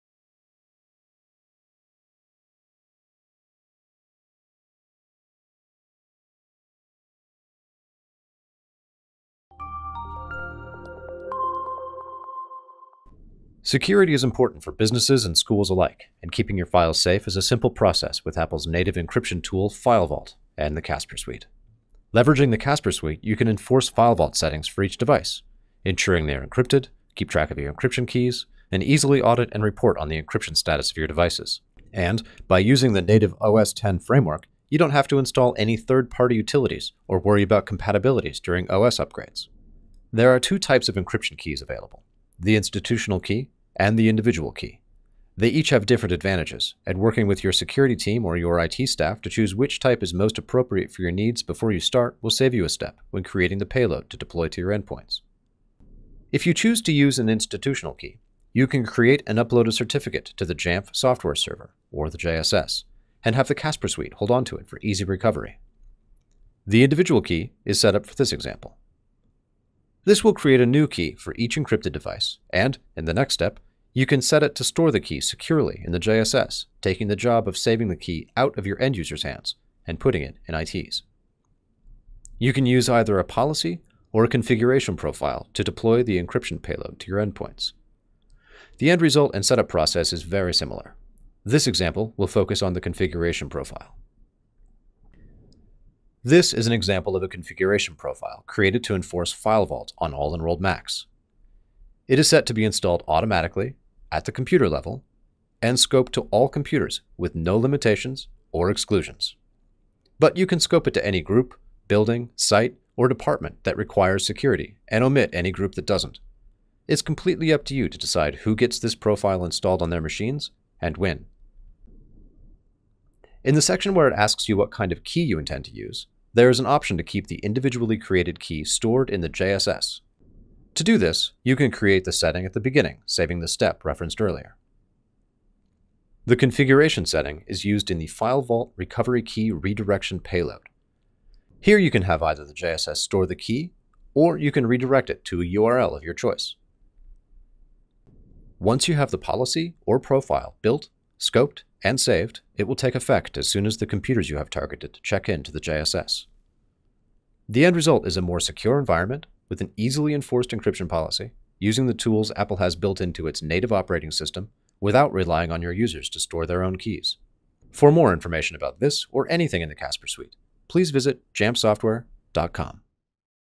FileVault for OS X and Jamf Pro
The need to keep data safe has pushed organizations to mitigate risk by encrypting client machines. To help with this, Apple offers an encryption tool called FileVault that ships with every Mac.

Watch this short video and learn how to:

- Enforce FileVault settings for each device
- Keep track of encryption keys
- Audit and report on encryption status